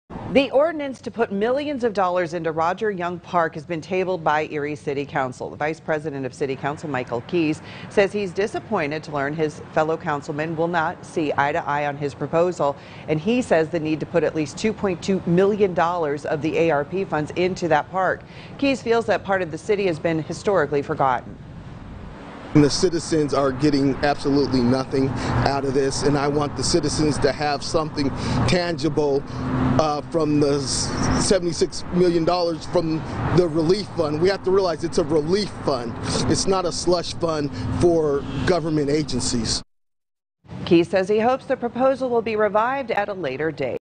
ARP funds on hold for Rodger Young Park
The ordinance to put millions of dollars into Rodger Young Park was tabled by the Erie City Council on Wednesday.